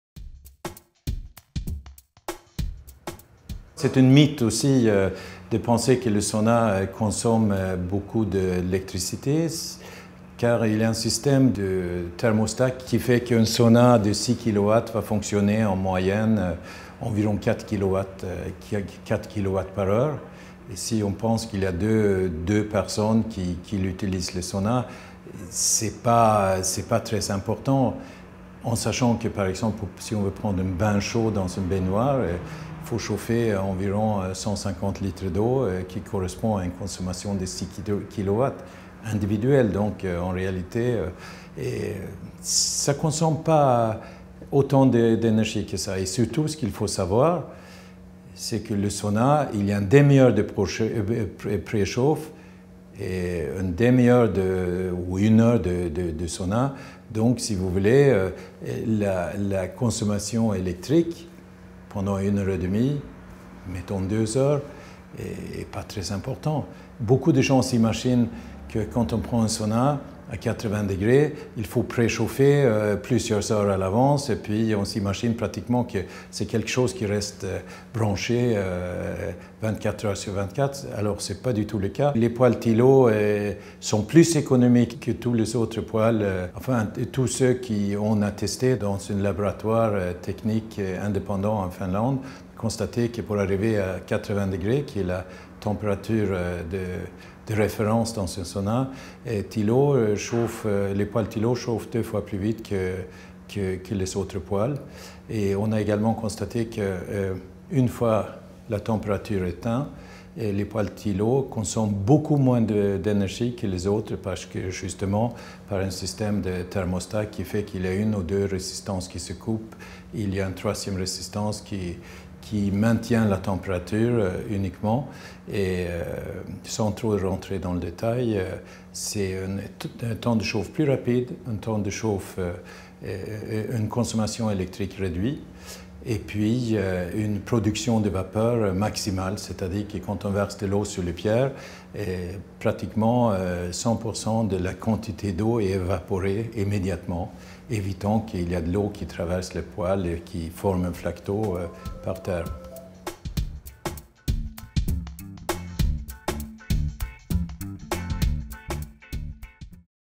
Nordique France - La consommation énrgétique d'un sauna
Votre consommation est fonction de la dimension de votre sauna; elle varie de 2 à 20 KW pour les cabines les plus grandes. Comptez environ 1,3 KW par m3 chauffé pour un sauna standard...